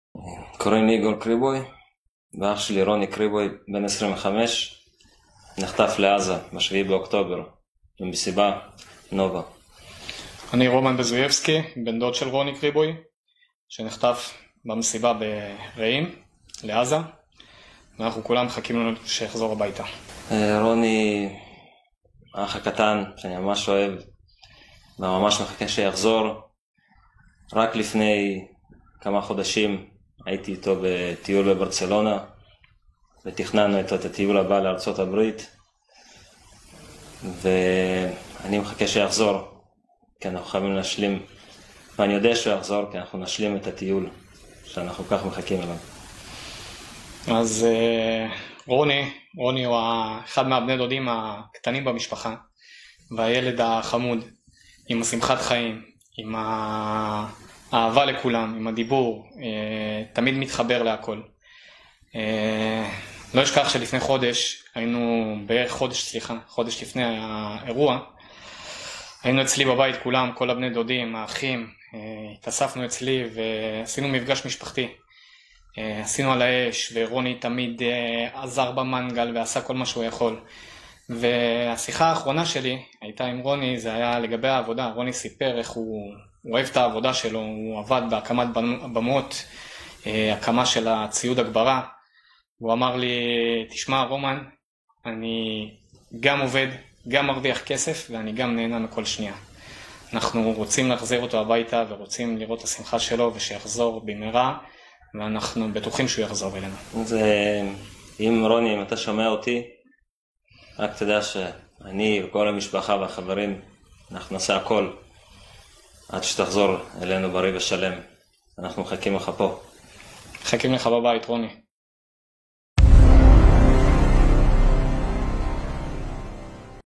רוני קריבוי, תחזור הביתה
משפחתו של רוני קריבוי שנחטף לעזה במסיבת הטבע בקיבוץ רעים, קוראת להחזירו הביתה, בריא ושלם. צילום: עיריית כרמיאל